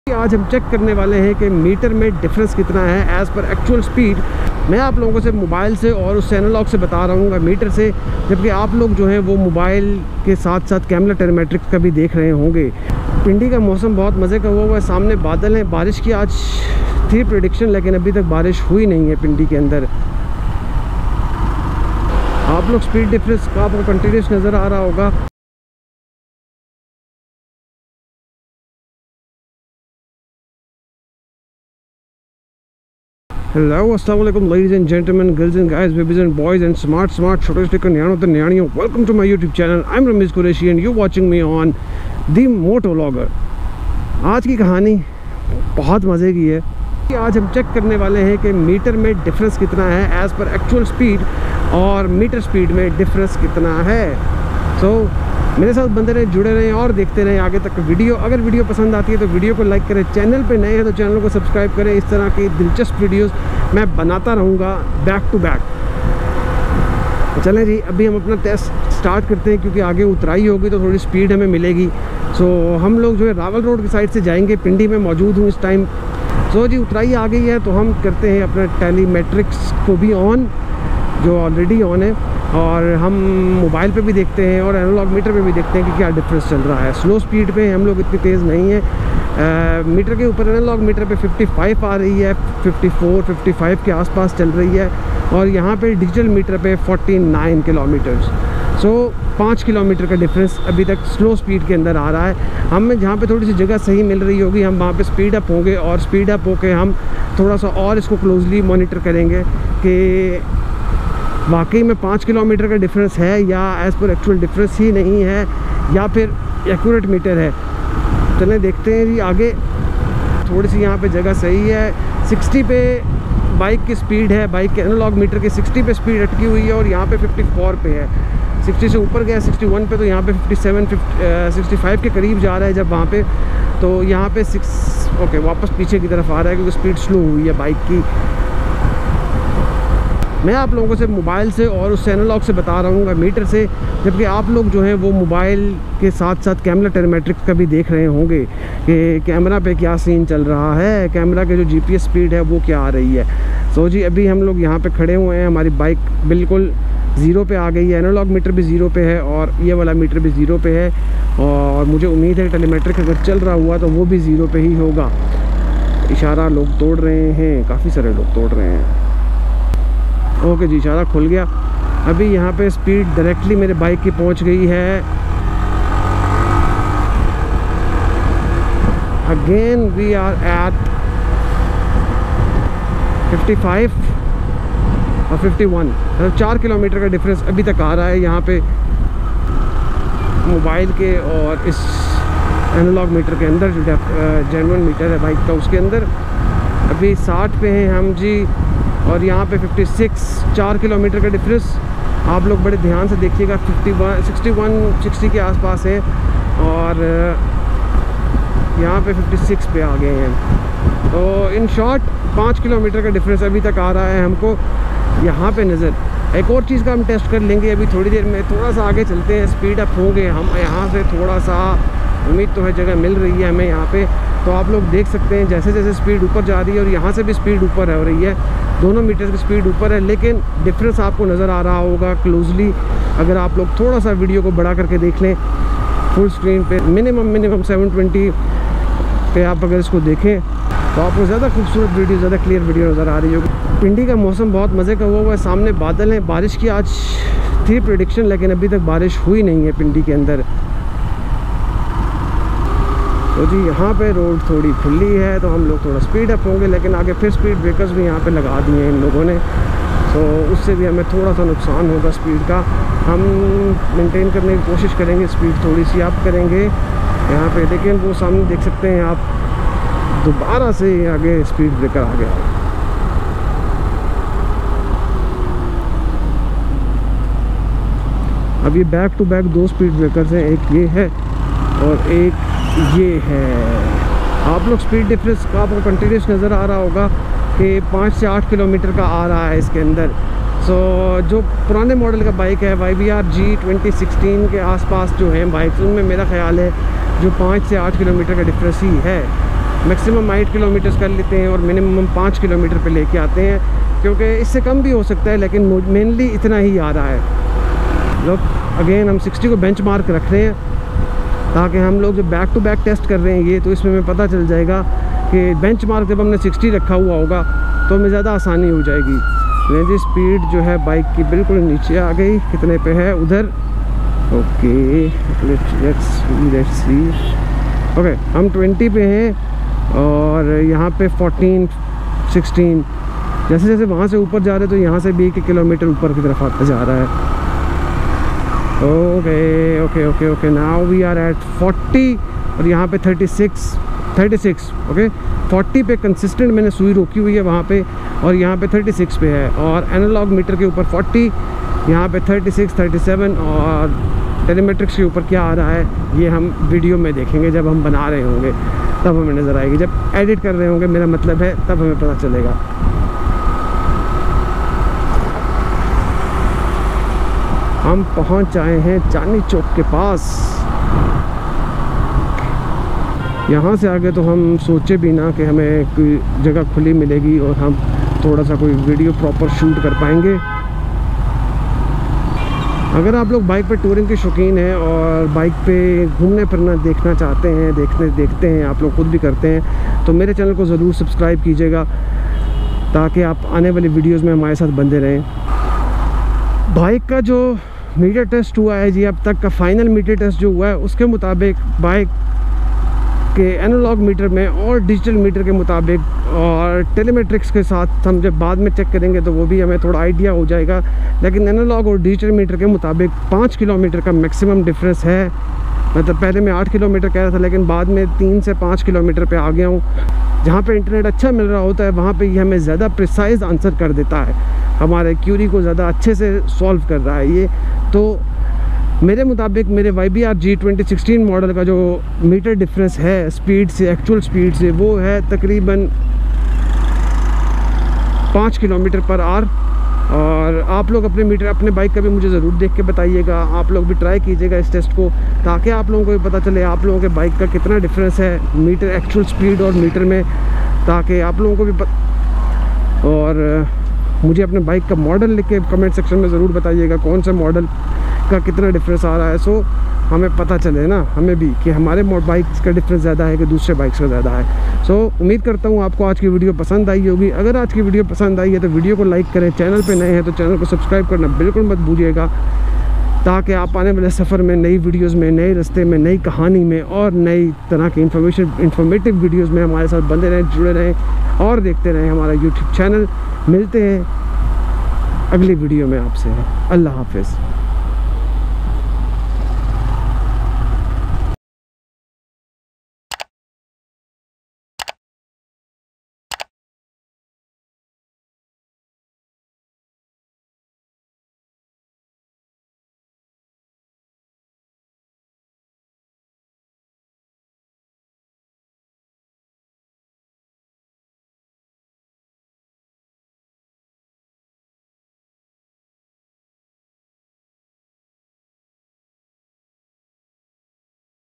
Yamaha YBR-G Meter Test | Best Tour Modification For Bikes | Digital Meter For YBR | Ramiz Biker
In this video we will be checking the meter difference between yamaha ybr 125 g orignal meter and digital meter from mobile app also will be putting effort to add more accuracy through gopro telemetric.